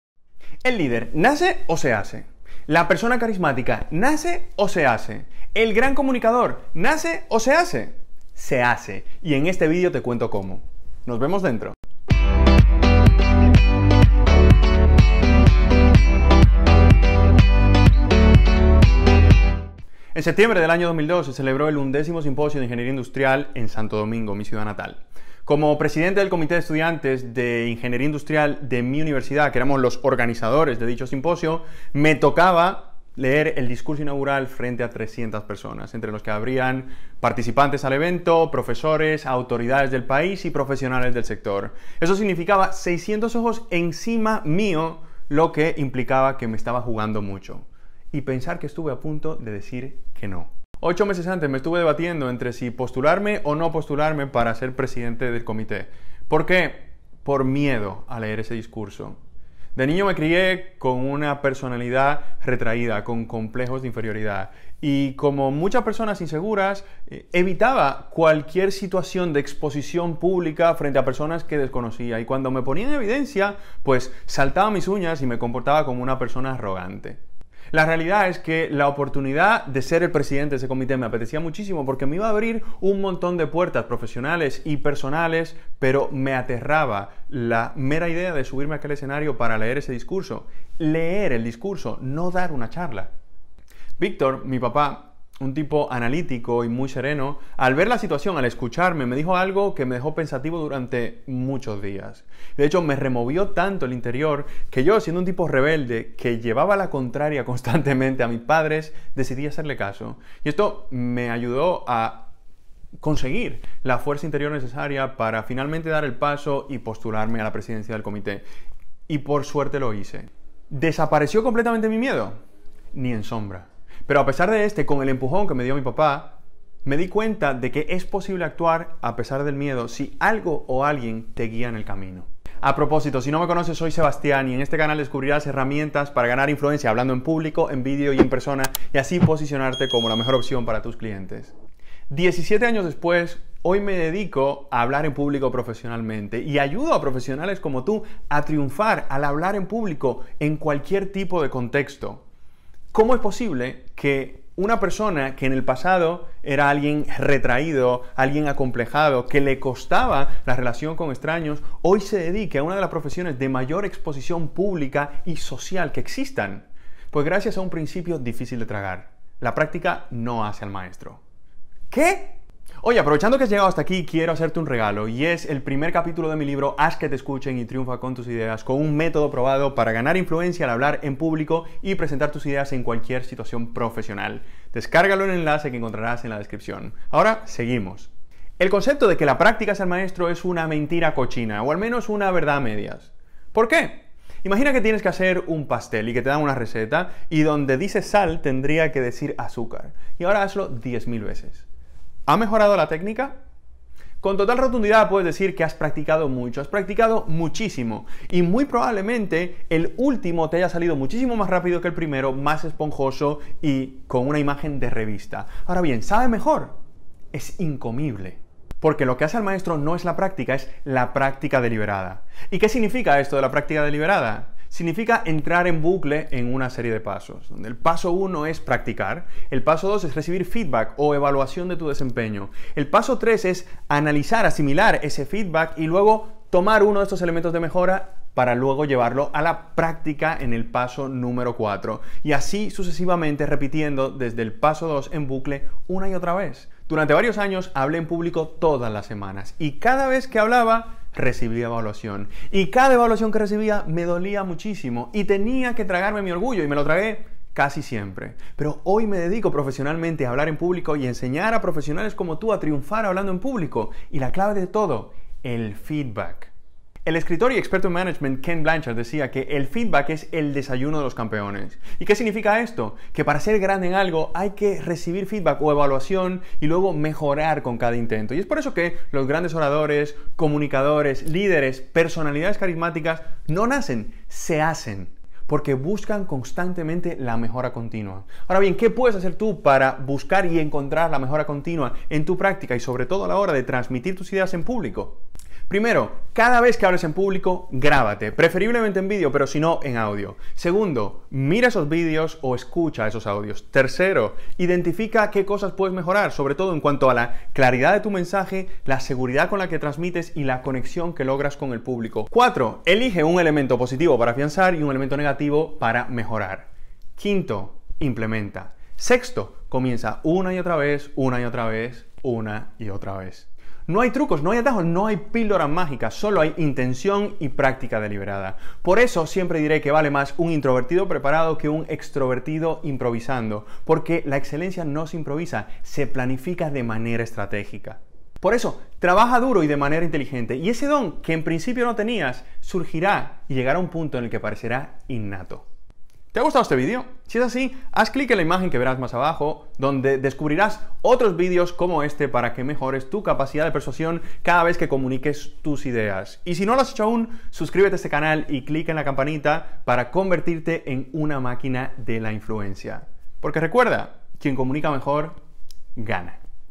Cómo Aprender Cualquier Cosa y Ser Bueno en Lo Que Sea | 6 pasos para hablar bien en público- Mito 3
🛑 Elimina los 5 mayores errores de comunicación en el trabajo (te están saliendo muy caros) ✅ Acceso especial a mi programa online Conecta y Véndete Mejor

¿Quieres saber cómo aprender cualquier cosa? Con estos 6 pasos para la excelencia descubrirás cómo ser bueno en lo que sea.